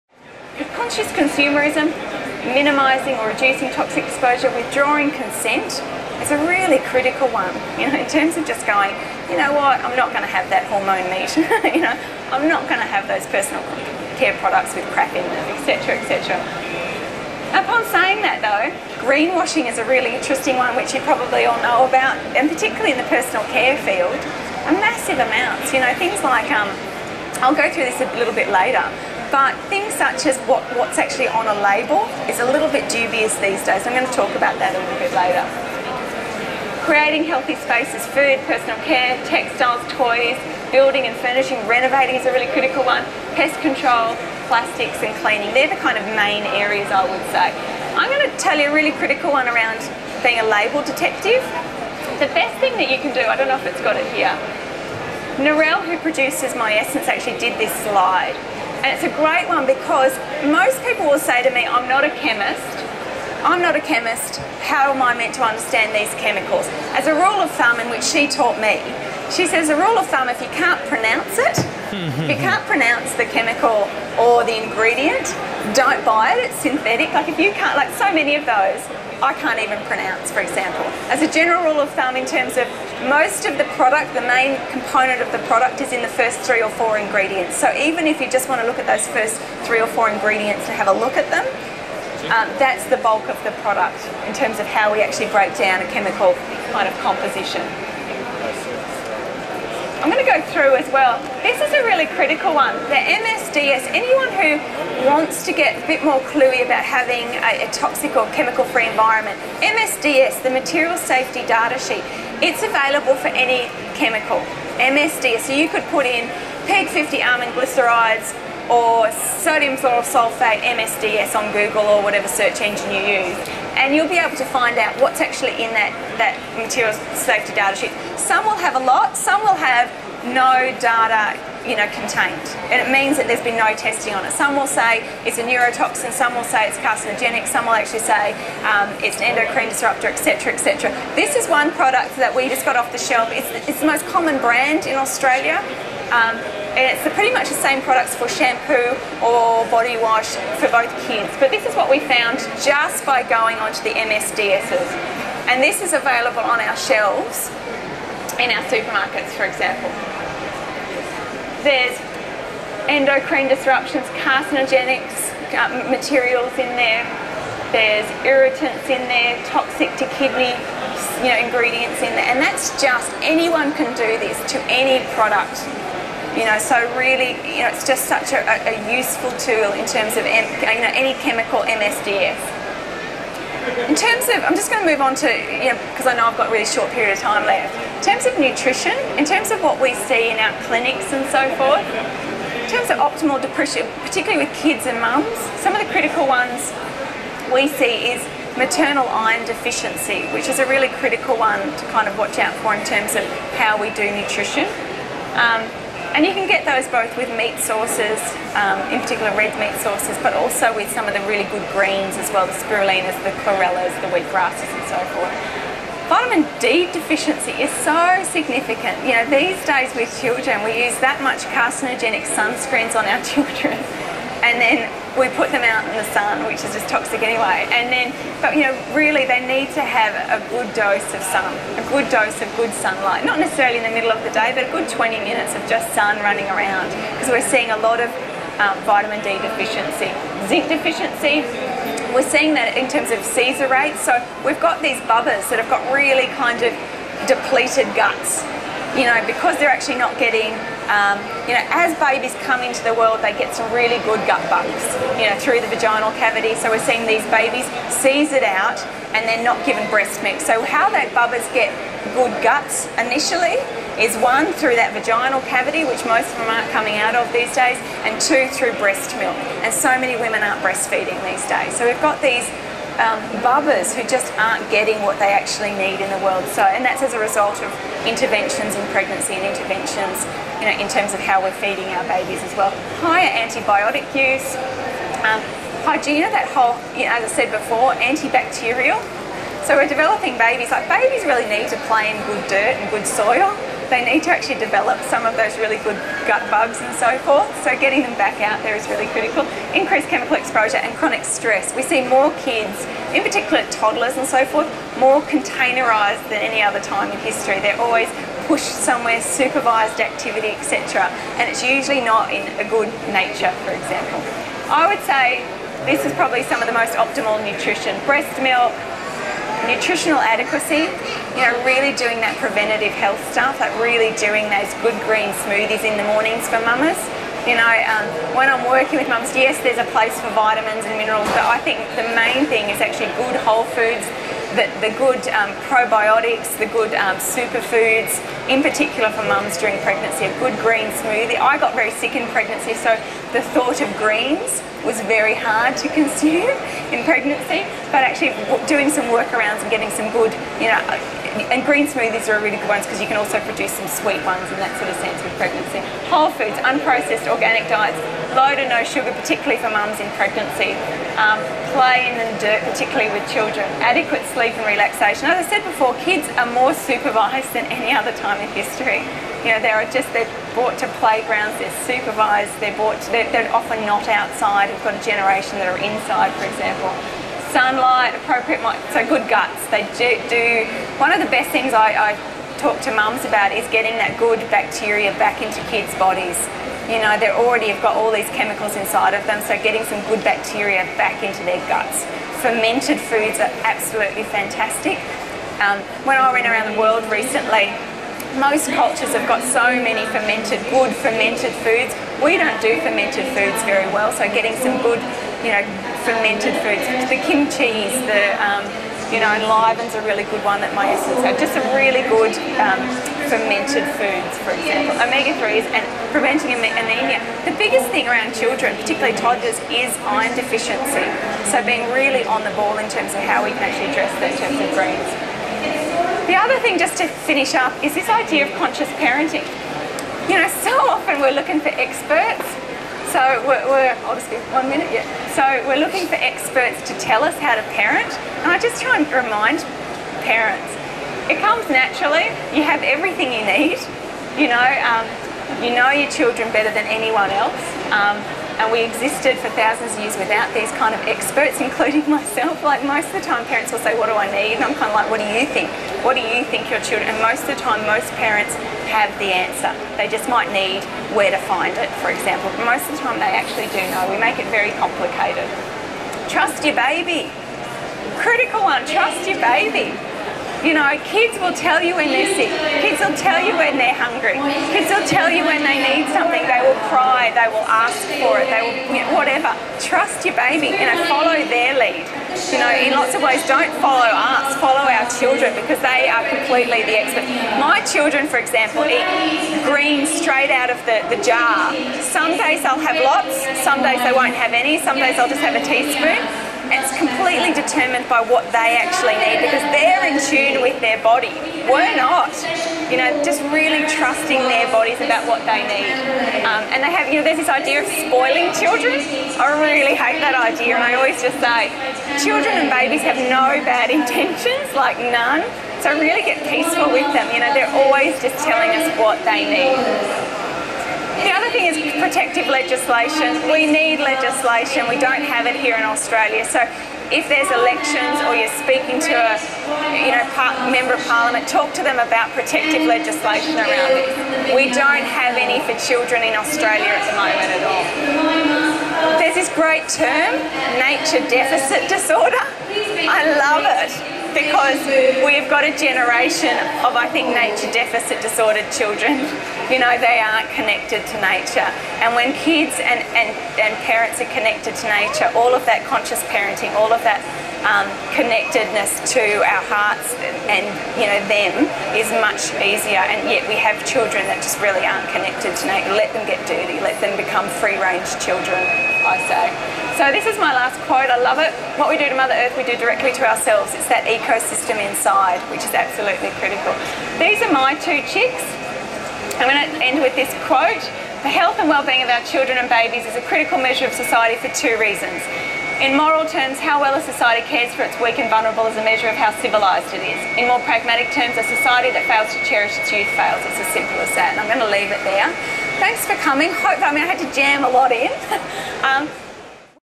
Chemical free kids part 5 // Dr Sarah Lantz // Organic expo & Green show international
In Part 5 Dr Sarah Lantz discusses what chemicals you can find present in a newborn baby at Organic expo & Green show international Aug. 2011.